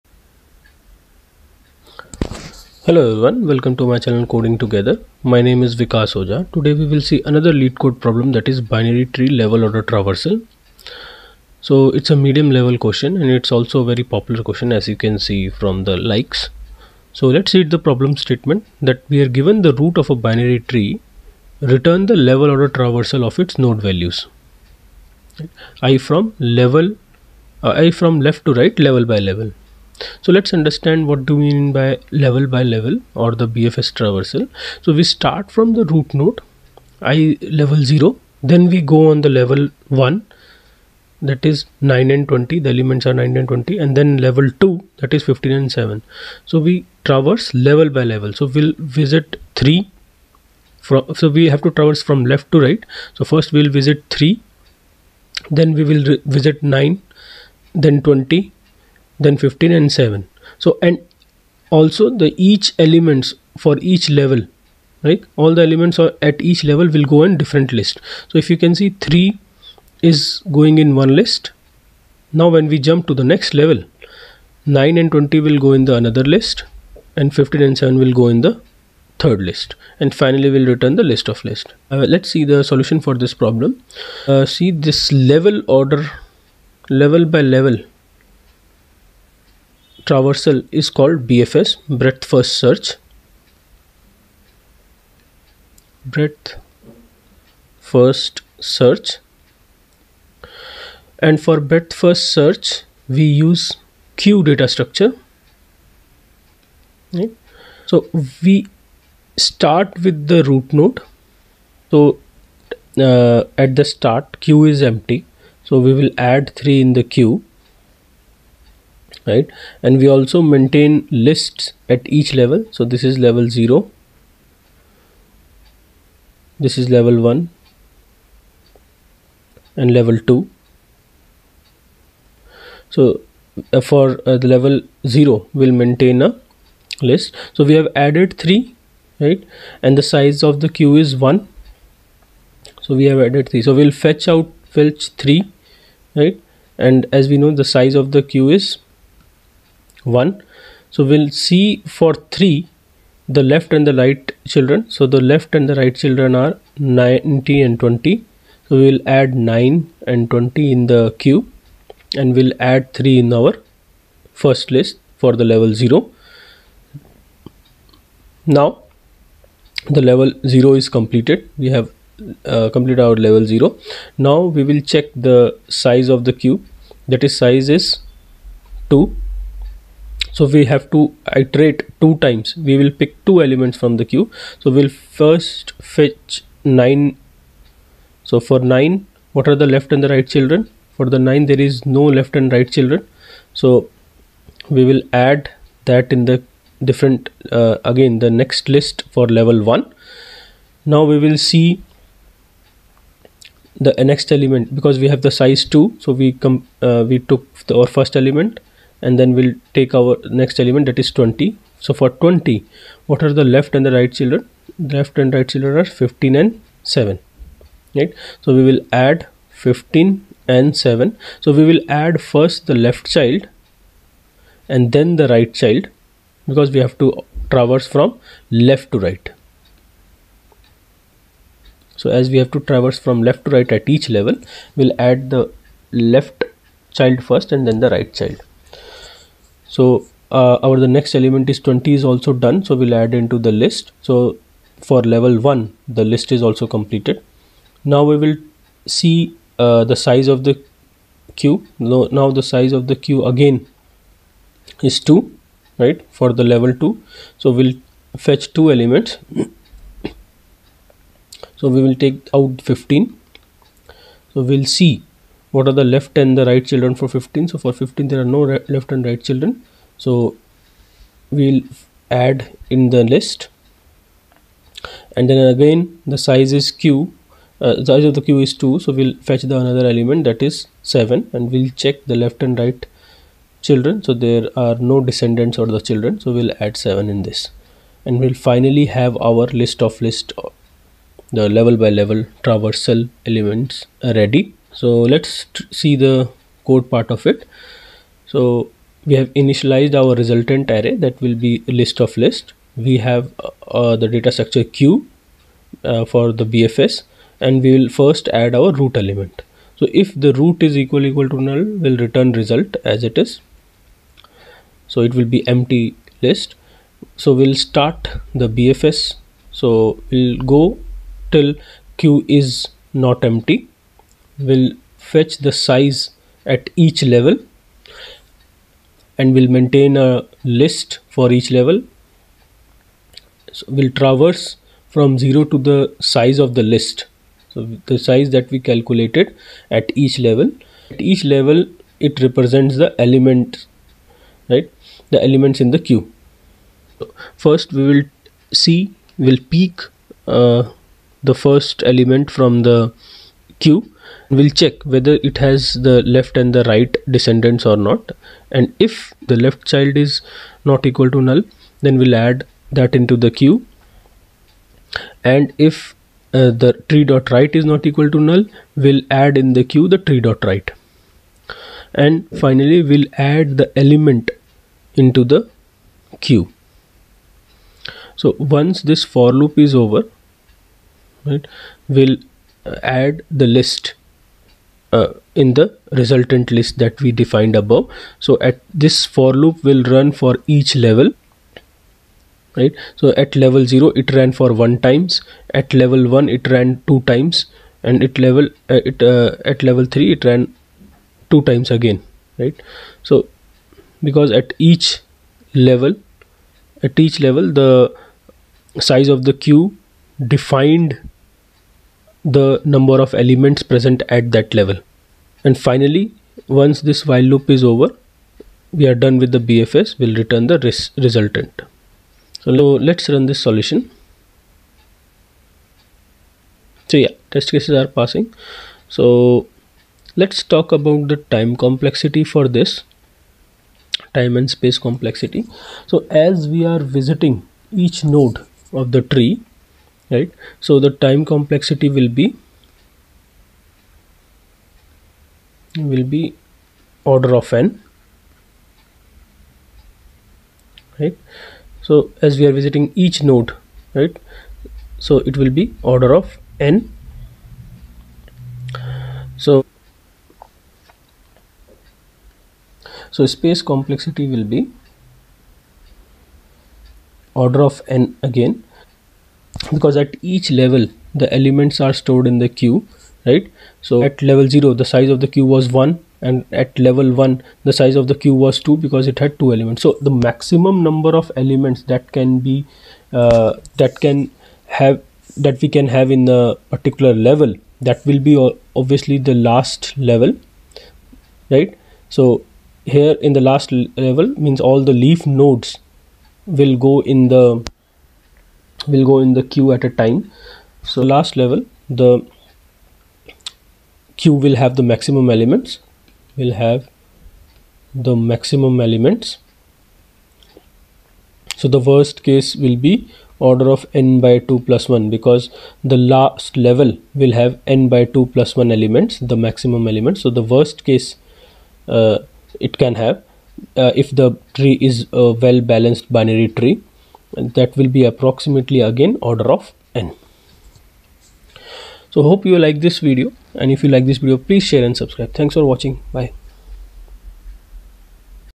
Leetcode 102 | Binary Tree Level Order Traversal | BFS
Here is the solution to "Binary Tree Level Order Traversal" leetcode question. Hope you like this video.